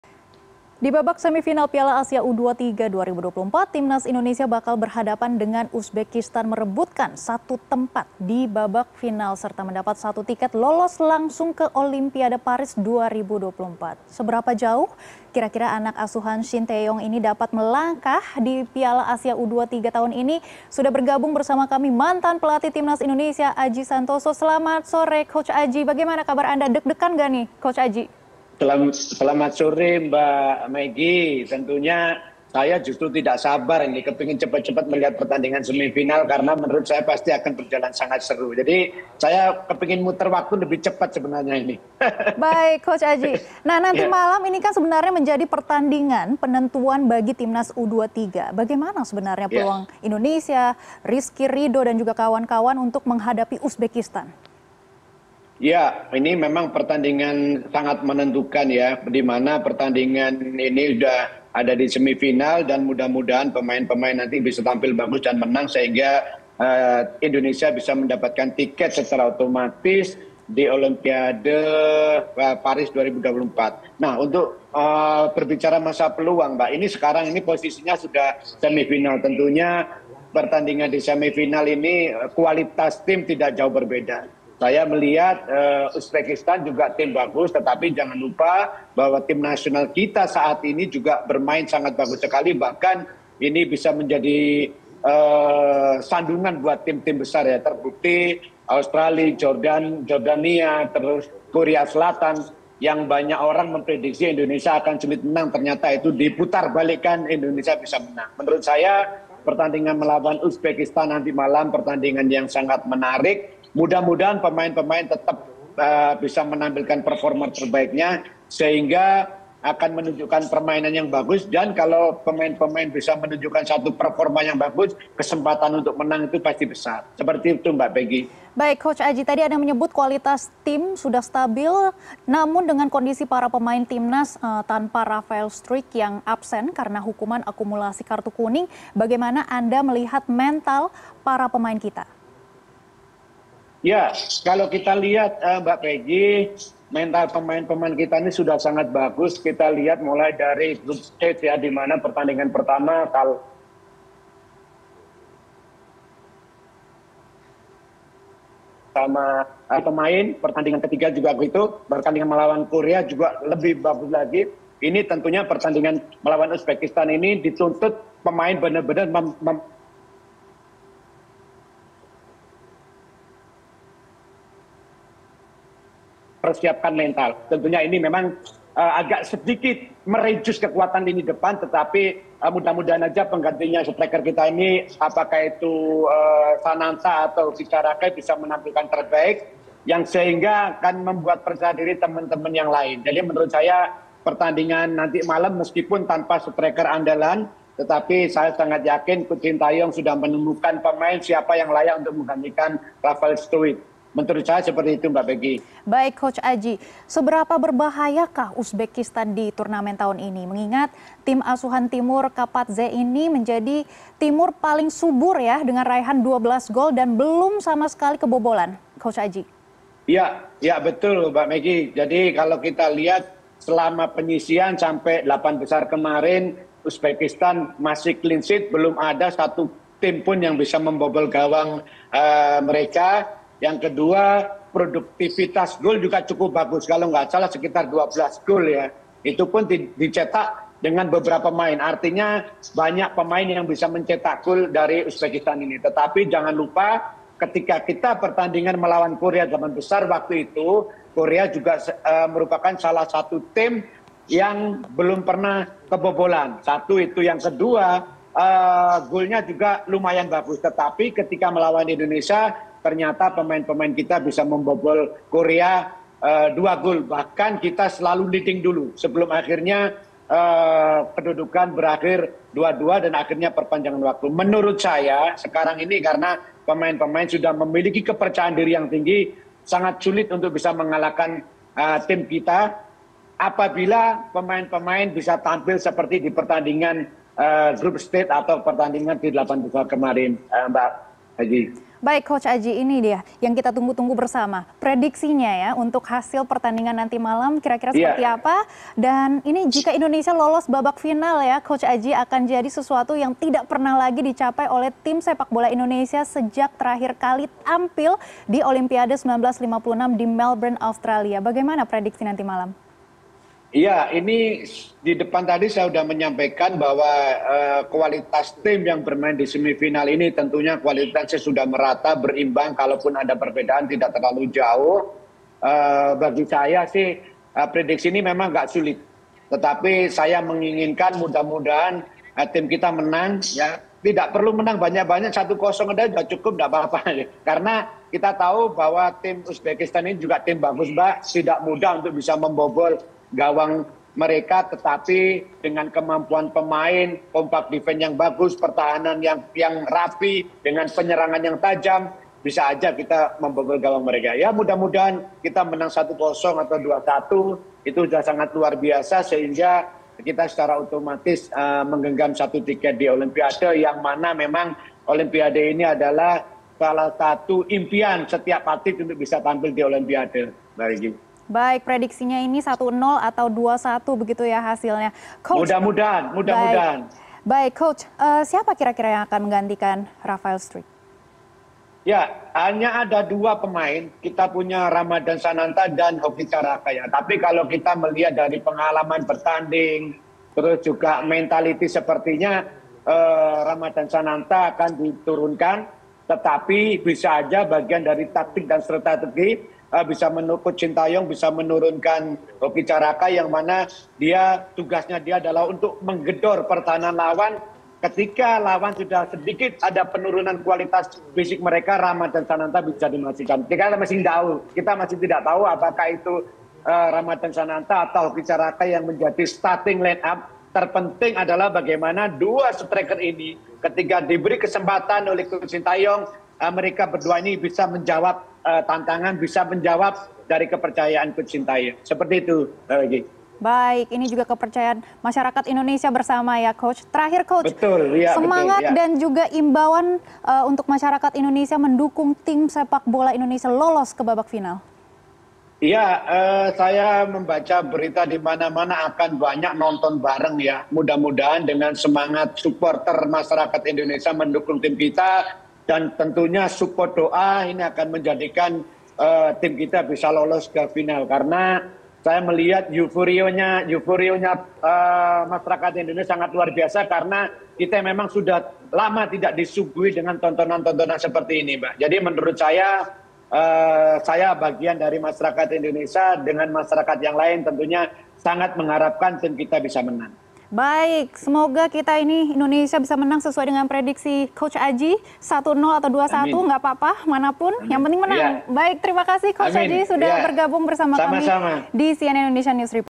Indonesia Siap Hadapi Uzbekistan di Semifinal
Di babak semifinal Piala Asia U-23 2024, Timnas Indonesia bakal berhadapan dengan Uzbekistan merebutkan satu tempat di babak final serta mendapat satu tiket lolos langsung ke Olimpiade Paris 2024.

Seberapa jauh kira-kira anak asuhan Shin Tae Yong dapat melangkah di Piala Asia U-23 tahun ini?
Sudah bergabung bersama kami, mantan pelatih Timnas Indonesia - Aji Santoso.